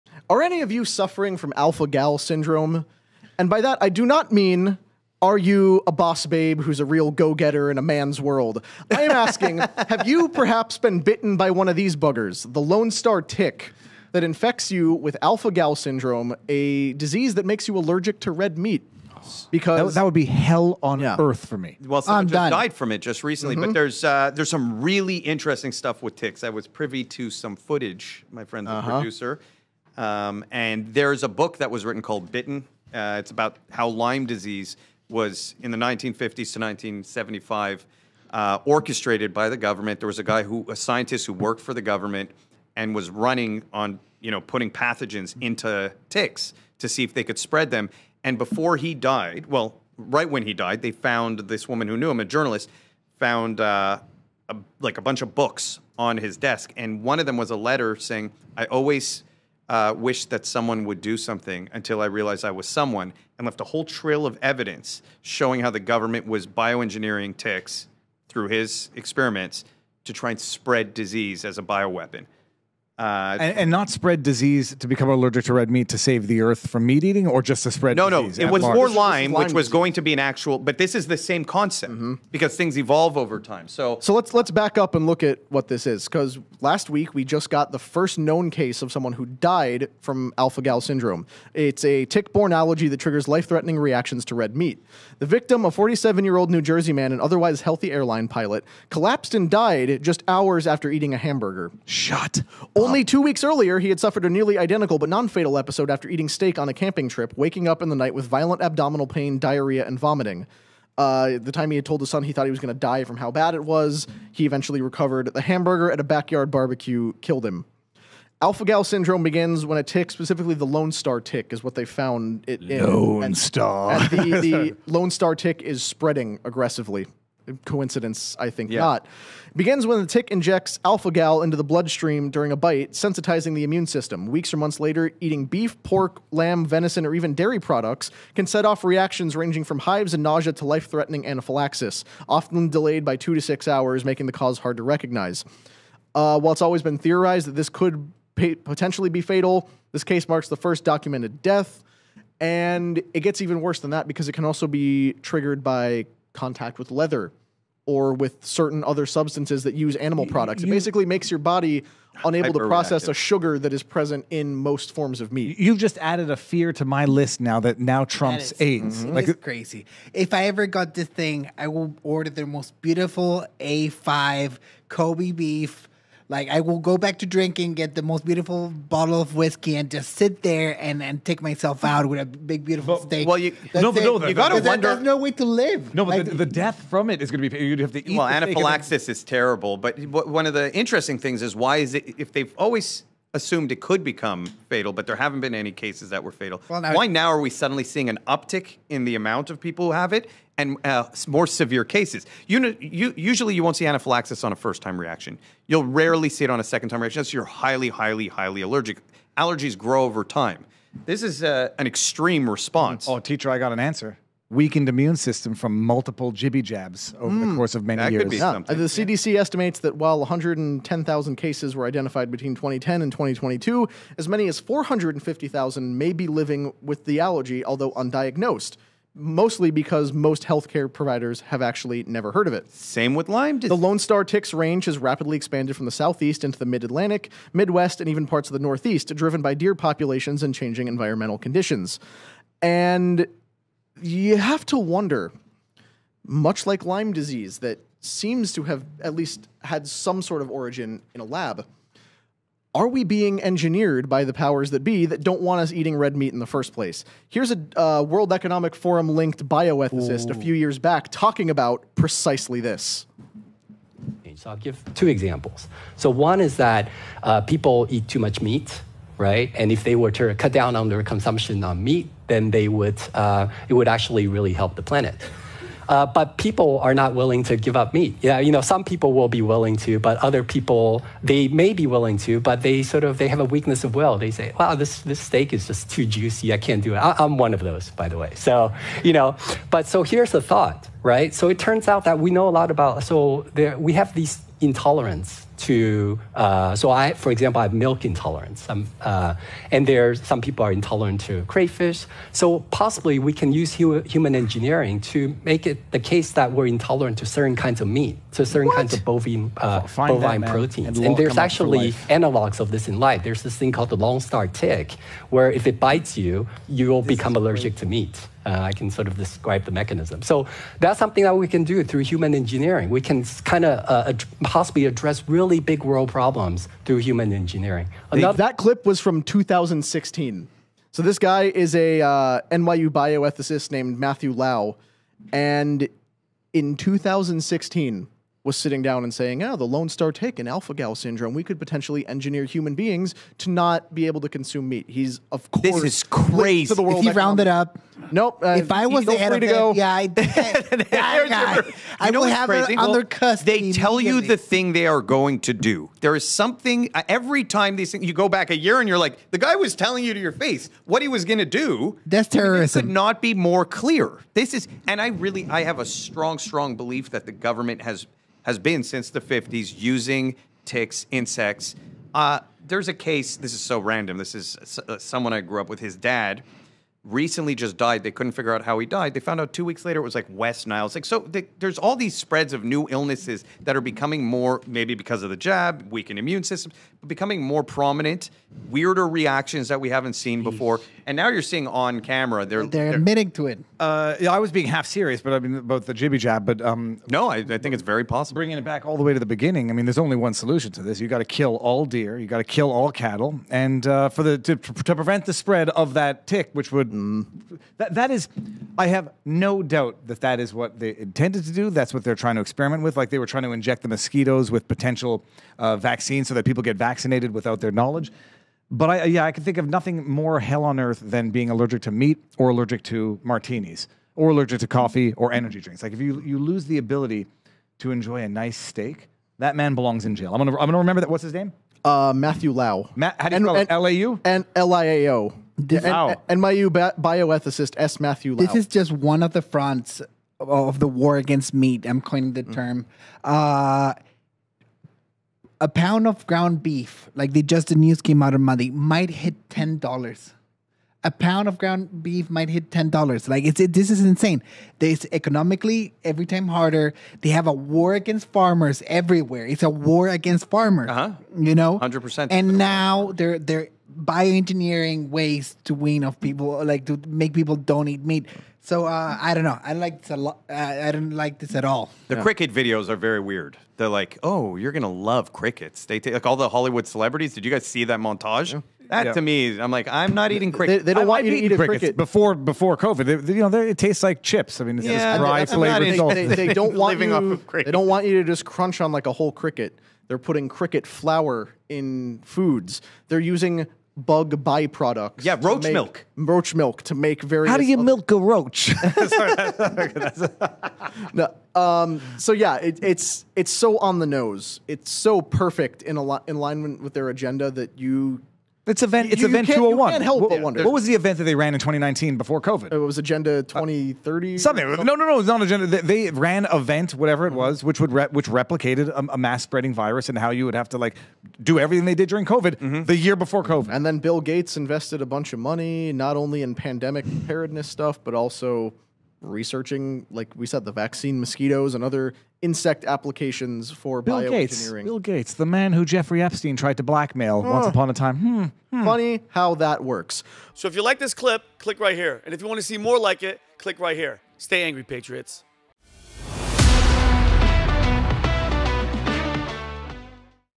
"Alpha Gal" Tick KILLS MILLIONS With Unstoppable Outbreak
A deadly case of Alpha-Gal syndrome has shocked doctors and raised urgent questions about the exploding spread of the Lone Star tick. Today, we breakdown how the meat allergy works, why cases are rising, and what makes this condition so hard to diagnose.